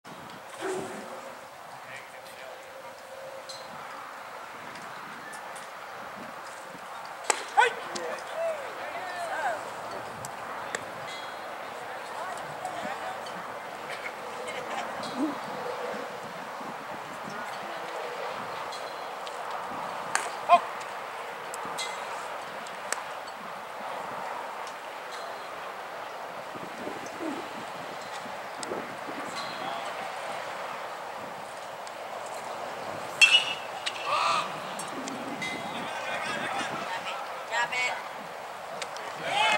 JOSH CORDOVA 2020 KEIN COLLINS LHP
JOSH CORDOVA 2020 KEIN COLLINS LHP @ Lamar University

Cordova showed an advanced slider for a 2020 graduate. He also featured a FB and CB.